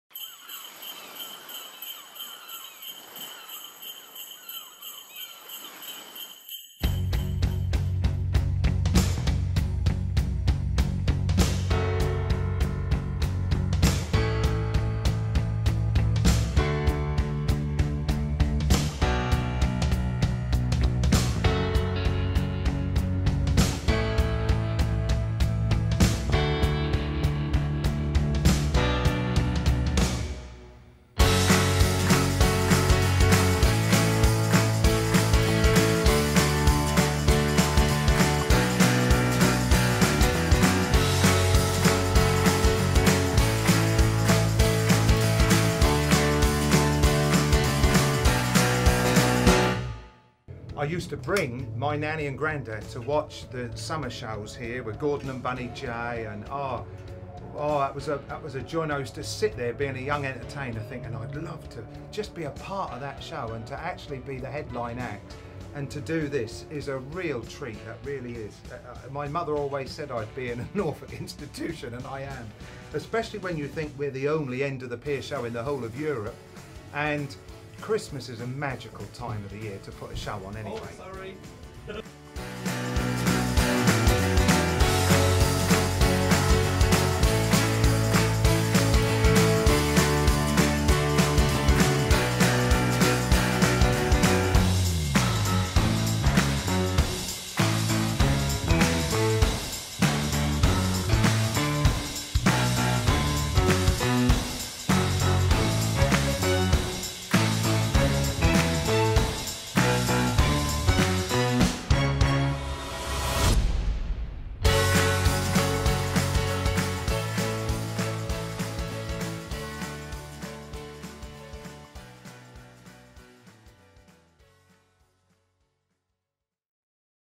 The Cromer Pier Christmas Show 2015 (Seaside Special)
The famous Cromer Pier Show, now the ONLY 'End of the Pier' Show of it's kind in the world, returned for its 11th Christmas Show in 2015. The show starred local all round entertainer Olly Day, Magician Steve Zebs, and singers Keith Pemberton and Lulu Alexandra, along with our famous Seaside Special dancers.